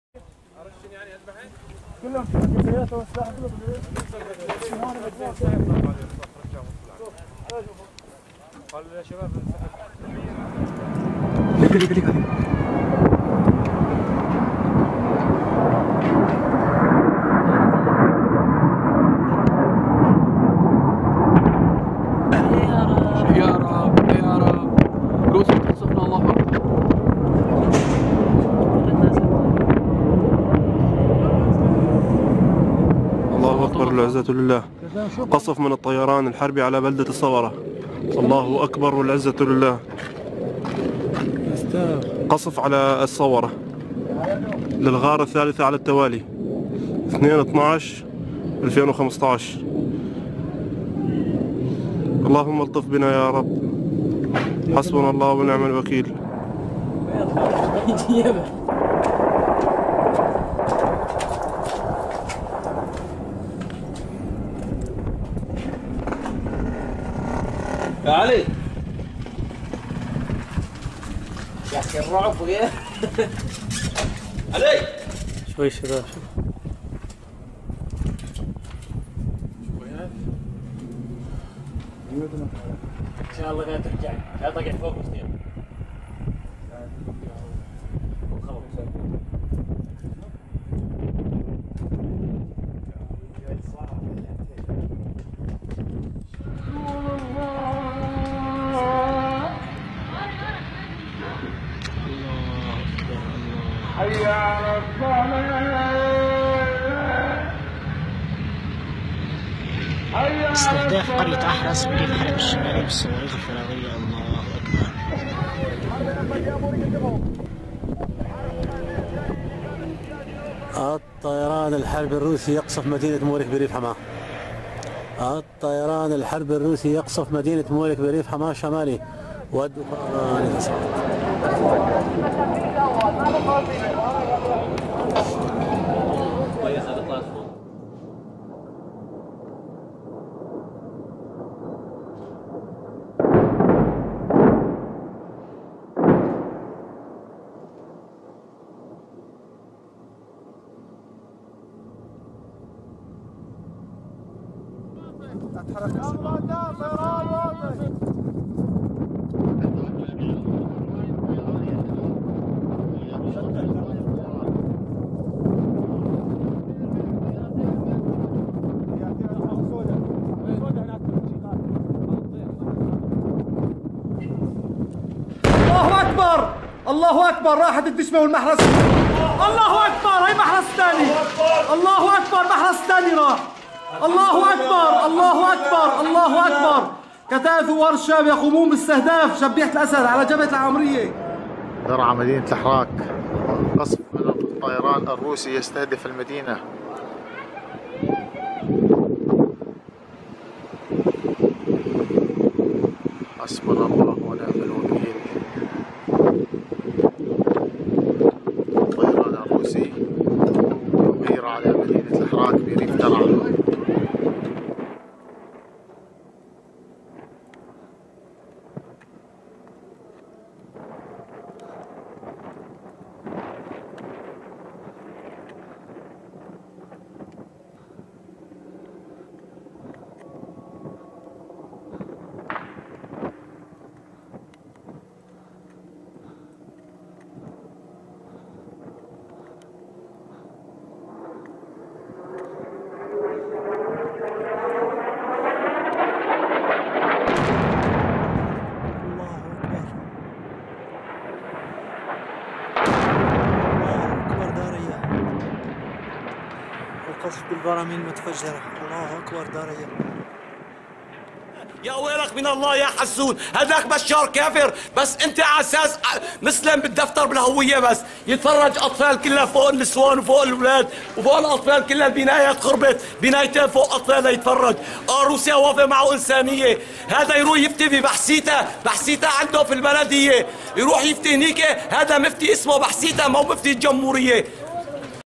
Russland bombardiert weitere Stellungen der Terroristen in Syrien 03 12 2015
Russland zerbombt weitere Stellungen der Banditen vom IS und von anderen Terrorgruppen in Syrien. Während dessen erzielt die Syrische Armee in der Bodenoffensive täglich Geländegewinne und schlägt die Banditen immer weiter zurück. Dass Geheule bei den USA und ihren Marionetten, ist ein weiterer Beweis für den Erfolg Russlands im Kampf gegen internationale Terrorgruppen, die von Westlichen Staaten und ihren Handlangern massiv unterstützt werden.